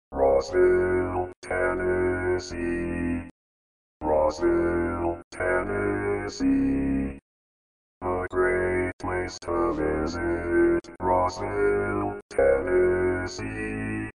How to Say or Pronounce USA Cities — Rossville, Tennessee
This video shows you how to say or pronounce Rossville, Tennessee.

A computer said Rossville, Tennessee. How would you say Rossville, Tennessee?